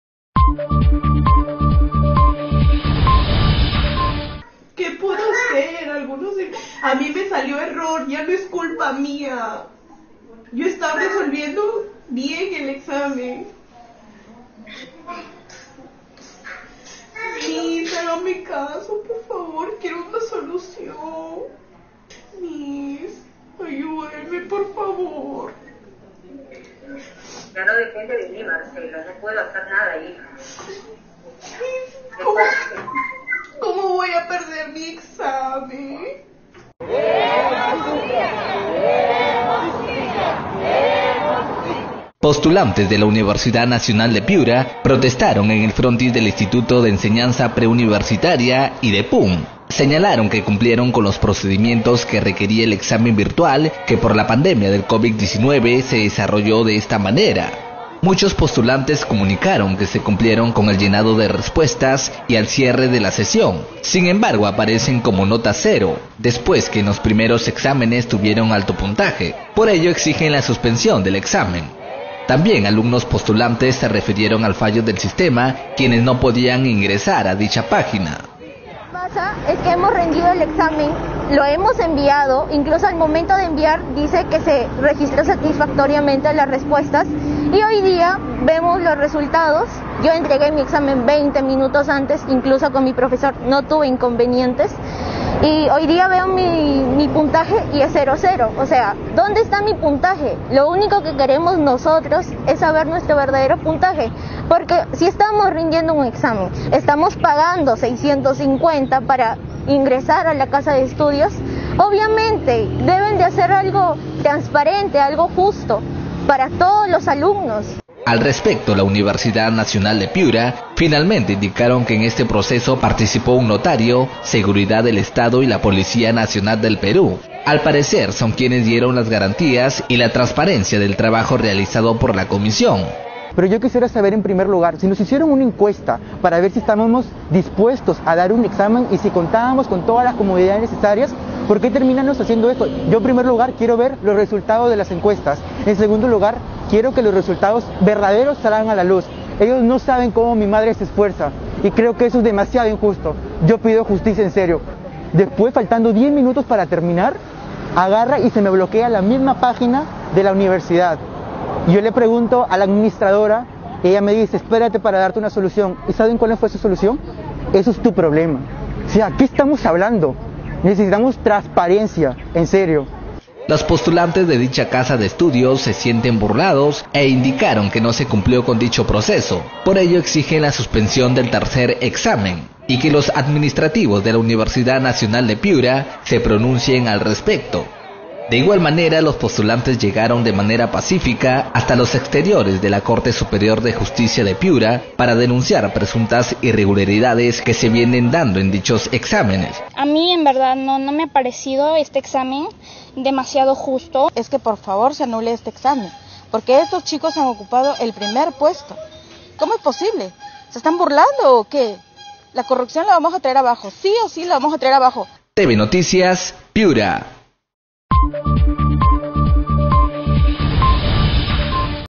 Piura - Postulantes a la UNP denuncian presuntas irregularidades en los exámenes de IDEPUNP
Postulantes a la universidad nacional de Piura, protestaron en el frontis del instituto de enseñanza pre – universitaria IDEPUNP, y denuncian presuntas irregularidades en examen, los postulantes a dicha casa de estudios, se sienten burlados e indicaron que no se cumplió con el proceso indicado por la Universidad Nacional de Piura, por ello exigen la suspensión del tercer examen, así lo solicitaron